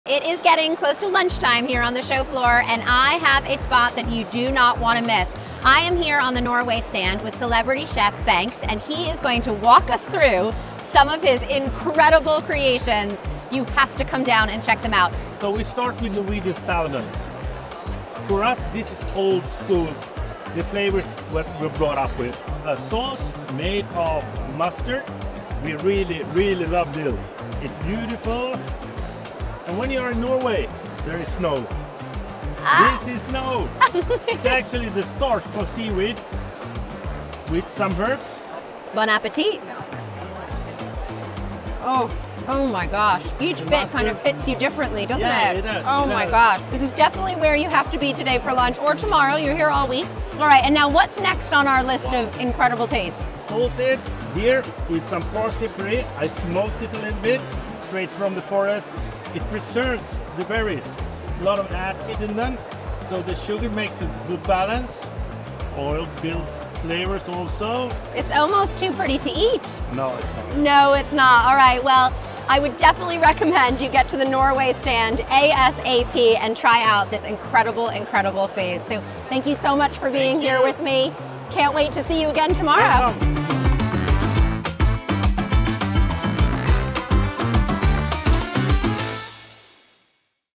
Bengt Sjöström takes us through some of his creative creations
Bengt Sjöström is serving up delicious dishes on the @visitnorway stand, stop by to try his creative creations today!